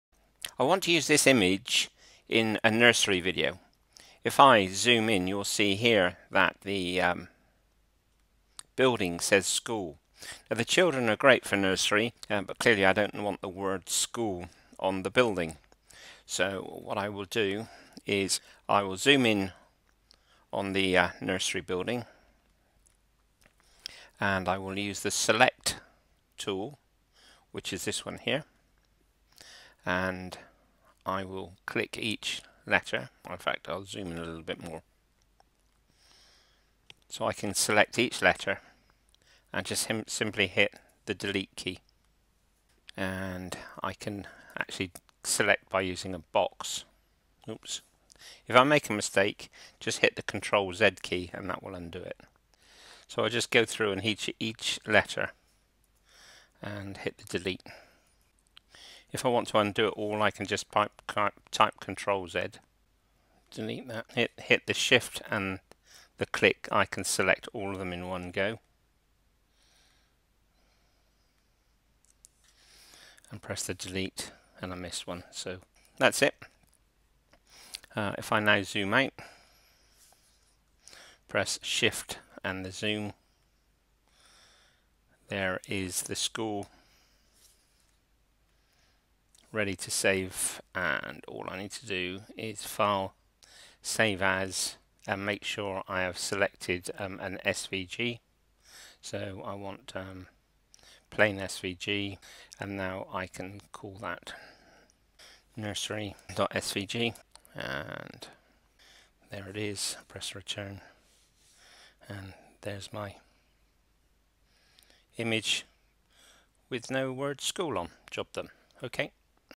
Inkscape Delete SVG Objects
Delete objects in an SVG image using inkscape